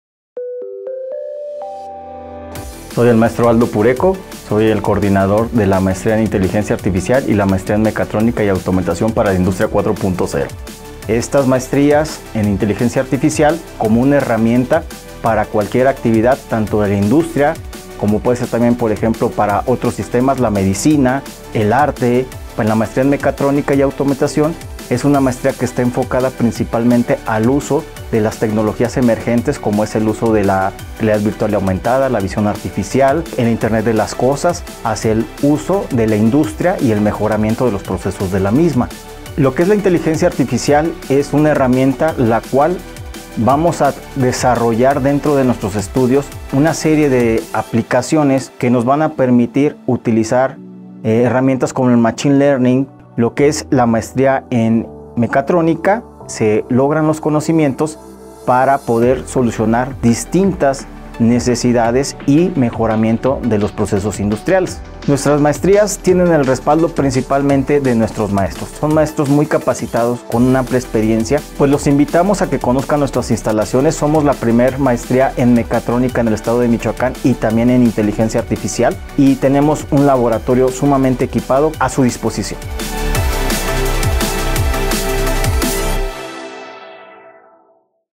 Aprovechan la ciencia y tecnología
La Universidad Vasco de Quiroga (UVAQ) ofrece tres posgrados en el área de Ciencia y Tecnología: Maestría en Ciencia de Datos, Maestría en Inteligencia Artificial y Maestría en Mecatrónica y Automatización para la Industria 4.0, opciones para convertirse en experto de las nuevas tendencias en ingenierías, señaló el Maestro Aldo Pureco, Coordinador de dichas maestrías.

Más información de estos posgrados y de otras áreas del conocimiento en la página